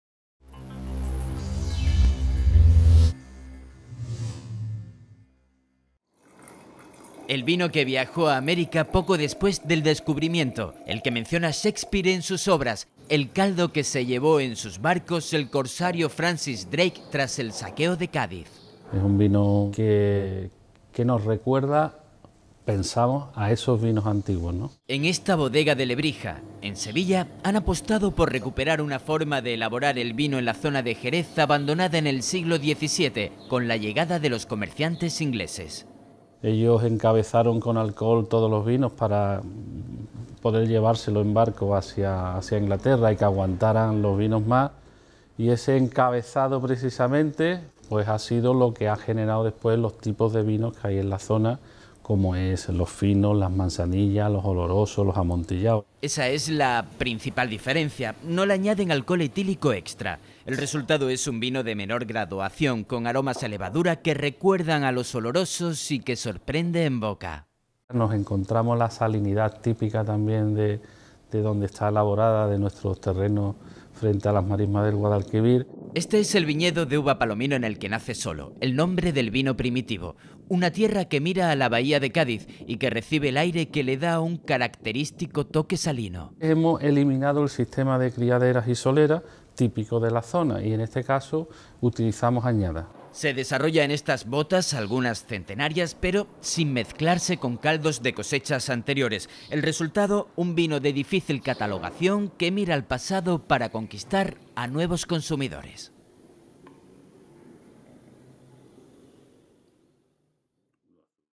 HDL Una bodega recupera el vino de Jerez que convenció a Shakespeare
En Lebrija, provincia de Sevilla, Bodegas González Palacios ha recuperado el vino de Jerez que viajó a América poco después del descubrimiento, el que menciona Shakespeare en algunas de sus obras. La llegada de los comerciantes ingleses a Jerez a partir del siglo XVII cambió en la zona la forma de elaborar los caldos. Y es la que se ha mantenido hasta nuestros días en la producción de finos, manzanillas y olorosos. La bodega lebrijana ha decidido volver a las formas primitivas y brinda la oportunidad de probar un vino similar al que bebieron los conquistadores de ultramar. El resultado, un vino blanco de uva palomino, de menor graduación, con toque salino en boca y de difícil clasificación.